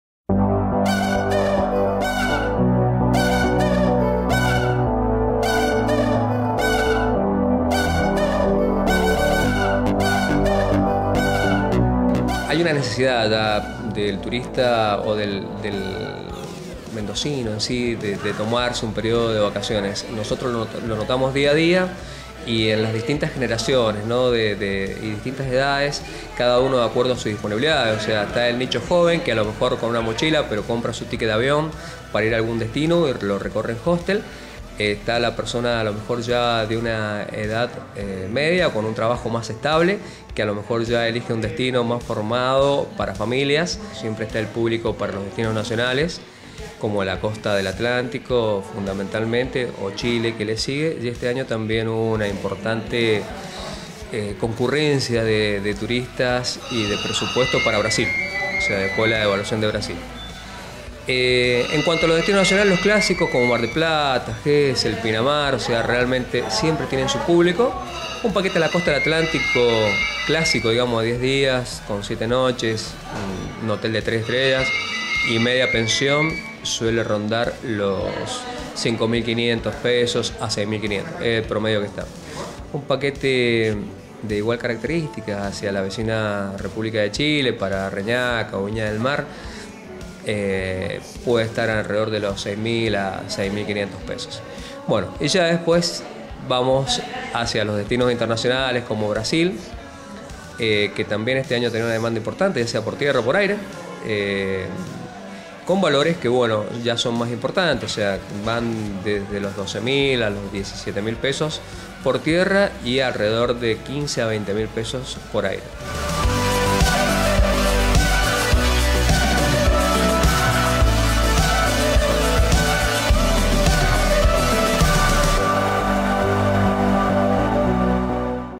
Principales destinos turísticos para los Mendocinos
¿Cuáles son los principales destinos turísticos que eligen los Mendocinos a la hora de pensar en sus vacaciones?